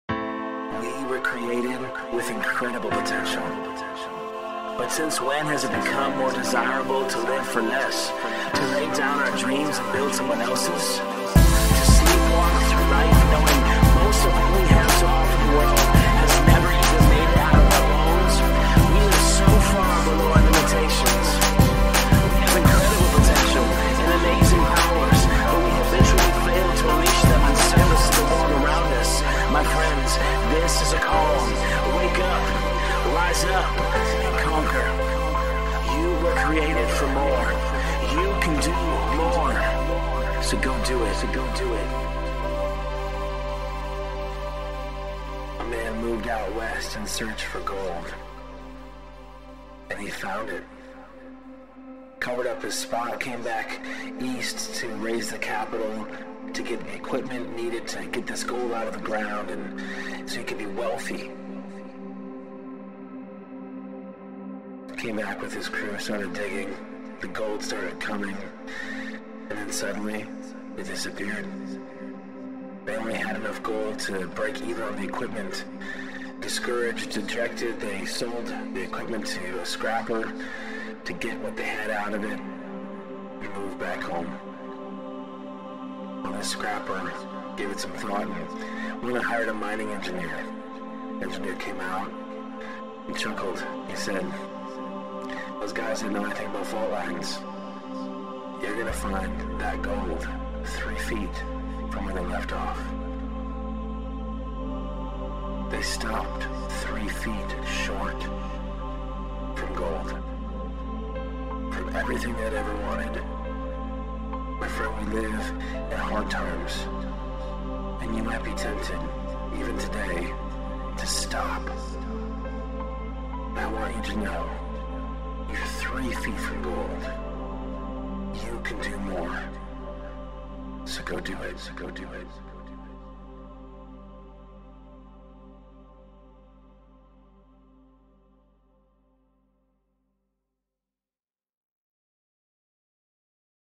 3ft from Gold
A man moved out west in search for gold.

And he found it.

Covered up his spot and headed back east to raise the capital he needed to get this gold out of the ground.

So he could be wealthy.

Came back with his crew and started digging. And the gold started coming.

And then suddenly, it disappeared.

They only had enough gold to break even on the equipment.

Discouraged, dejected, they sold the equipment to a scrapper to get what they had out of it and moved back home.

Well, this scrapper gave it some thought and went and hired a mining engineer.

And this mining engineer came out and he chuckled and said:

"Those guys didn't know anything about fault lines. You're going to find that gold 3 feet from where they left off."

They stopped 3 feet short from gold.

From everything they had ever wanted.

My, friend, we live in hard times.

And you may be tempted today to stop.

And I want you to know:

You're 3 feet from gold.

You can do more.

So, go do it.